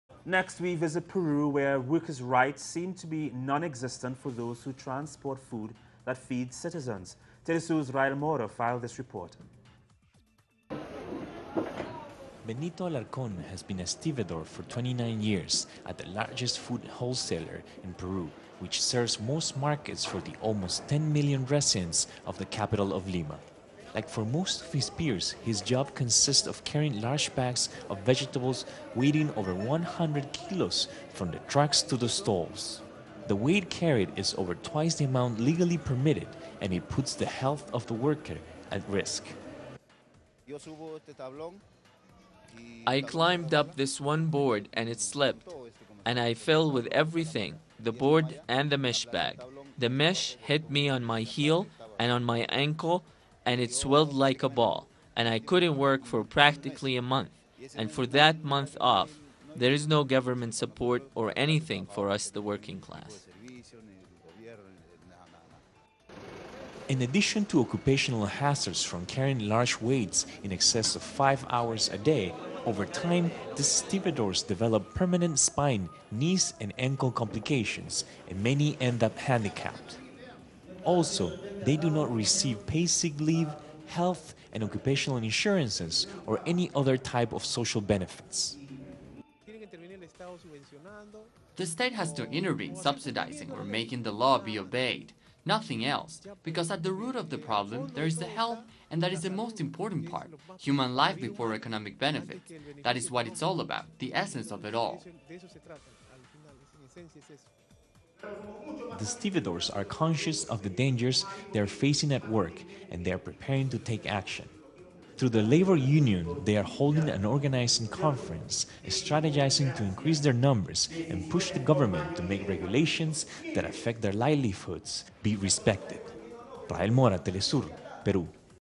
Peruvian Truck Loaders Work in Precarious Conditions
Peruvian workers who unload trucks at Lima's markets work in the most precarious of conditions. While receiving low pay, they suffer from chronic spine, knee, and ankle injuries as they have to carry loads of up to 100 kilos, twice the legal limit. In addition, they do not receive workplace benefits such as sick-leave or health care. Their union is starting to strategize and fight back for better working conditions. Rael Mora reports from Lima. teleSUR